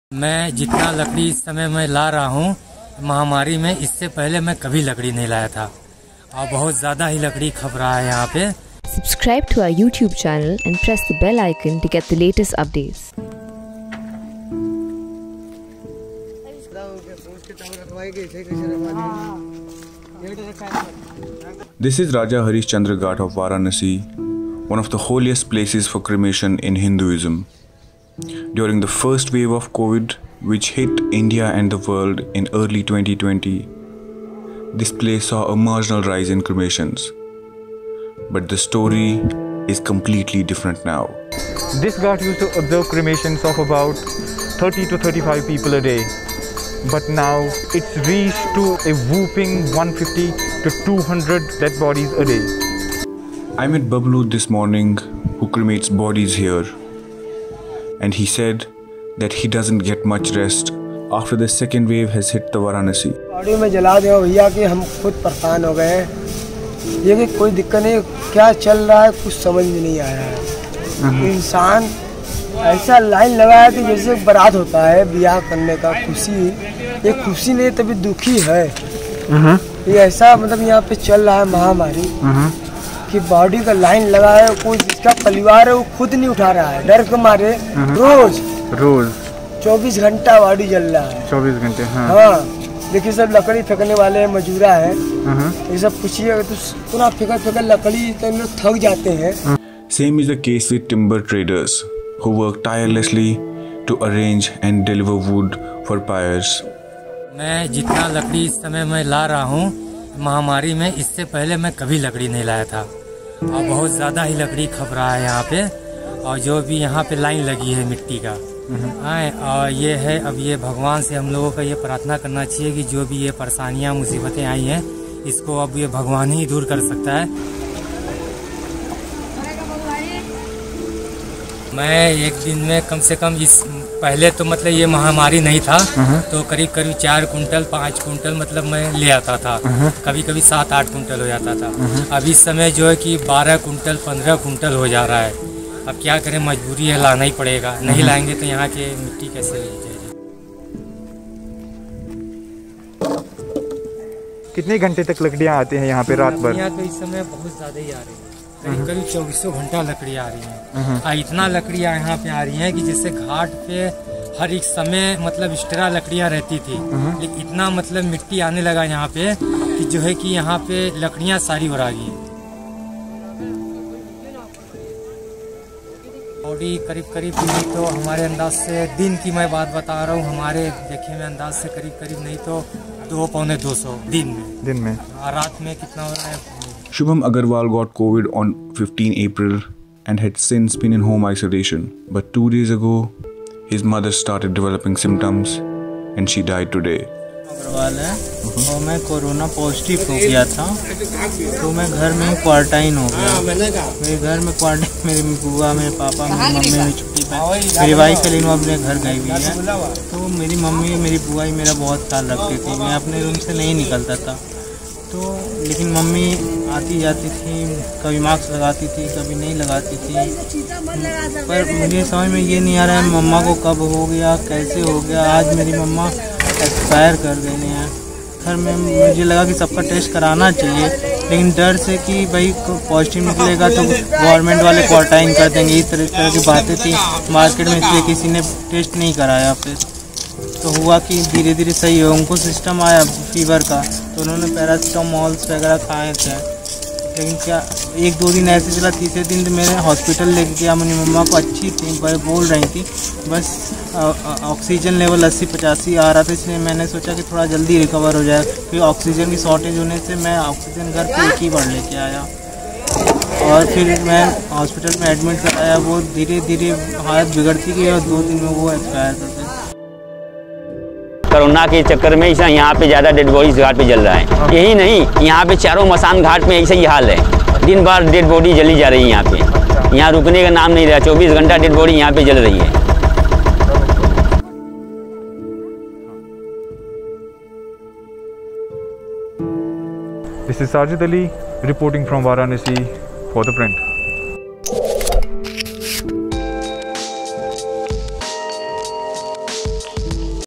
How Varanasi’s Raja Harishchandra ghat is coping with high number of cremations
The revered Raja Harishchandra Ghat of Varanasi used to see 20-40 Covid funerals a day during the first wave of the pandemic. However, this time, the number has increased to 150-200. ThePrint’s reports.